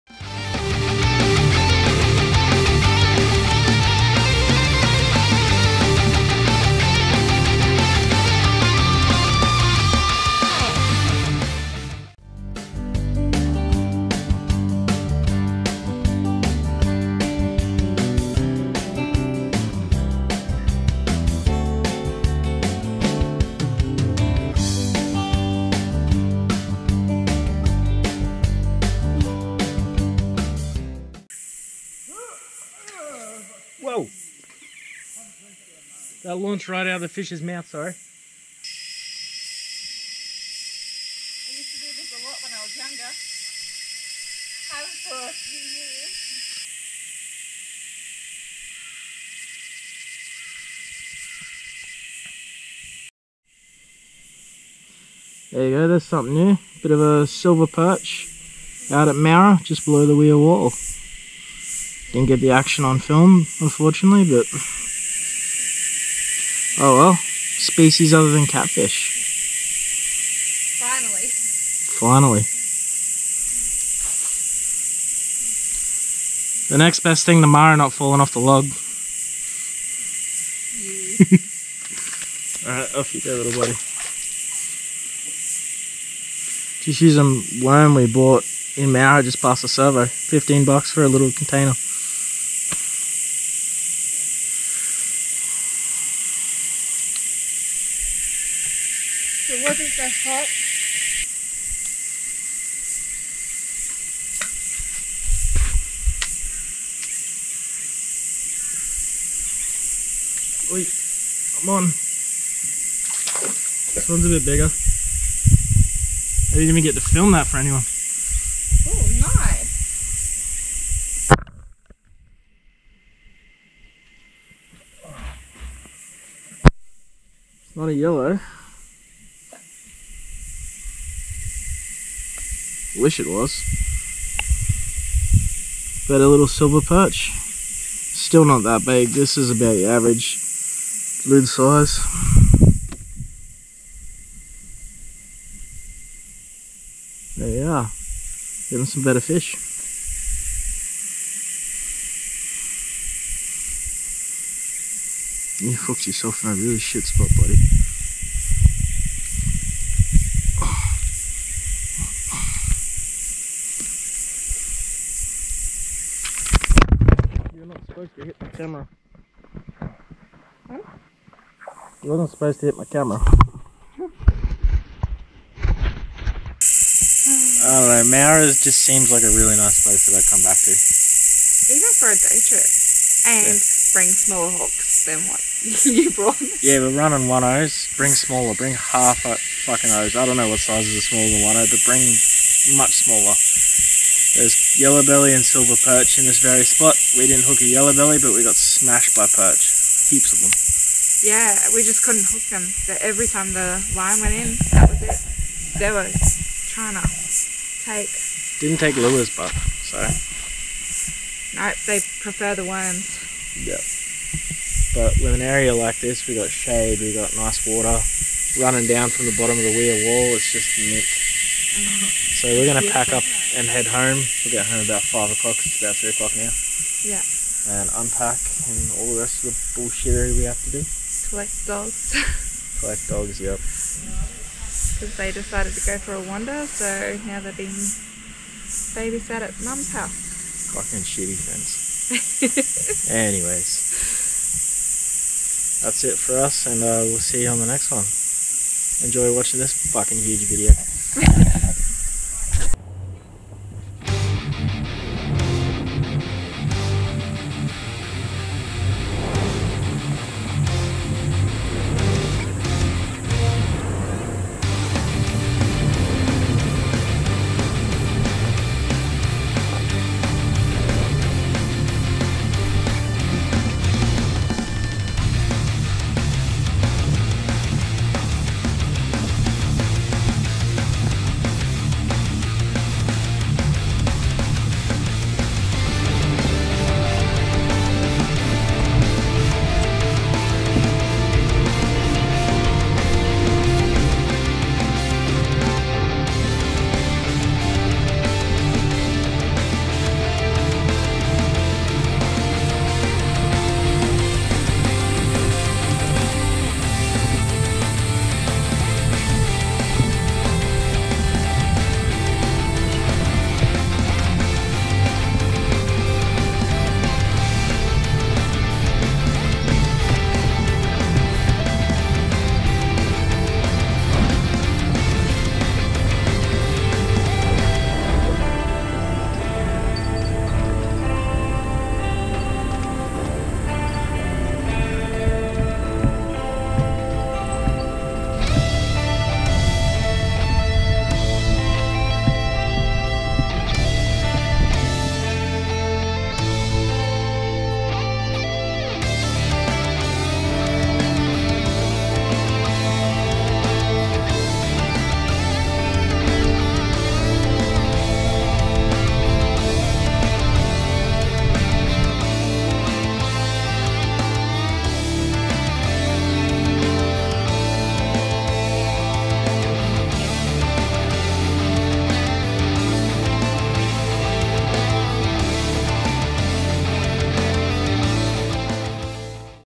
Fishless Fishermen!! Ep 31 Day 4!! Moura!!
a quick half day last attempt for yellowbelly in moura! somewhere we need to spend more time!!  Next Video its back to myself and rueben hitting the reefs at rat island!!